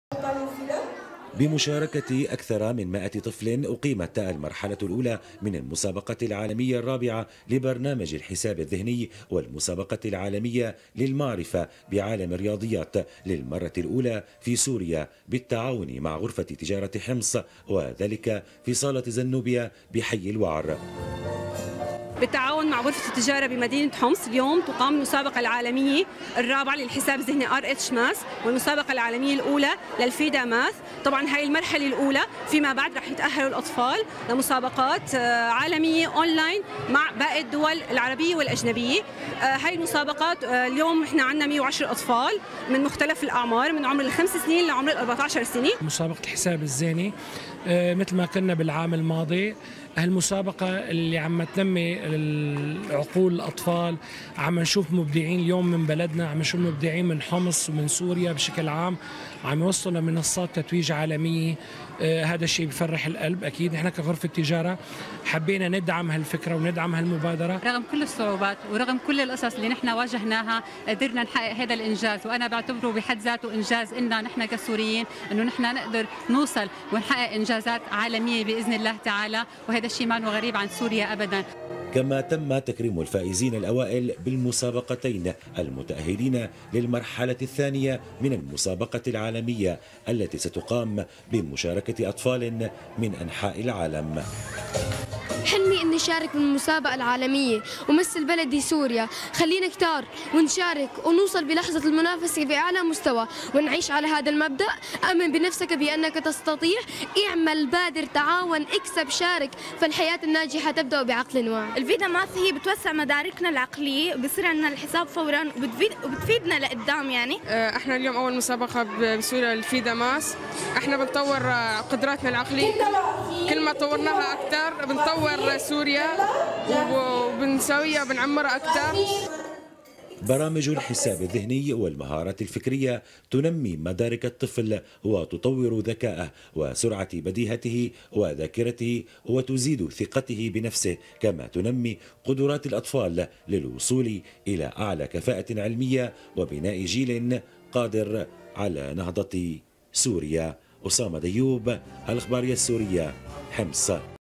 حمص | المرحلة الأولى من المسابقة العالمية الرابعة لبرنامج الحساب الذهني
تقرير اسامة ديوب